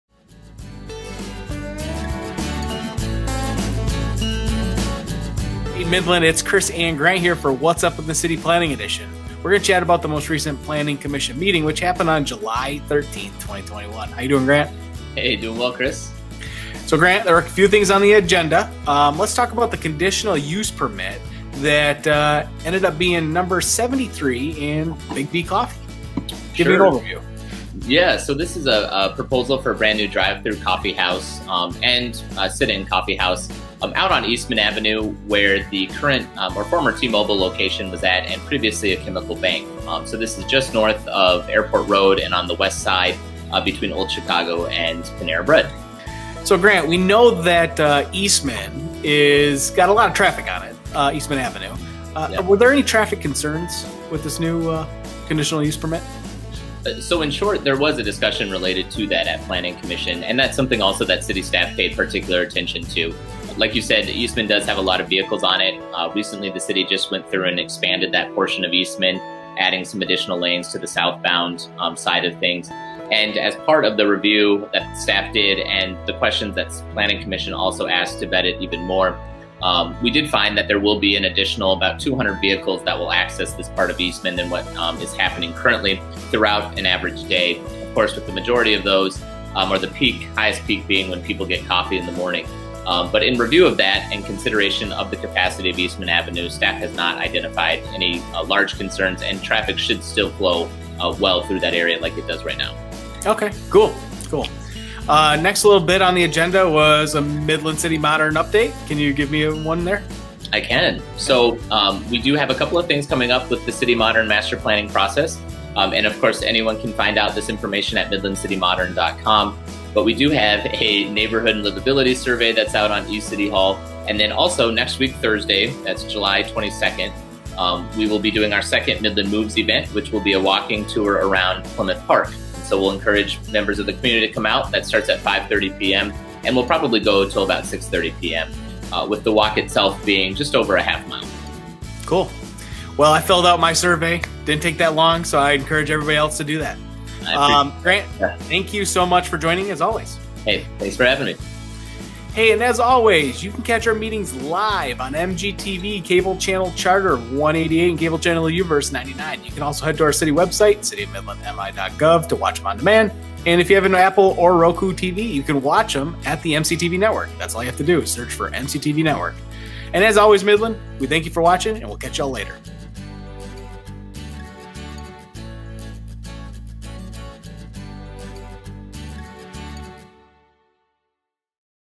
City of Midland | What's Up in the City! Planning: Biggby Coffee Coming to Eastman Avenue?
At the July 13, 2021 meeting of the Midland Planning Commission, Biggby Coffee received recommendation to City Council for a conditional use permit to open a new location at Eastman Avenue and Airport Road (formerly a T-Mobile and Chemical Bank location). With this recommendation, the City Council will set and hold a public hearing on the matter, with the final decision currently expected on Monday, August 9.

In this video, Kris and Grant also discuss upcoming events and a new survey opportunity as part of the City Modern Master Plan process.

To view the full July 13, 2021 Planning Commission meeting,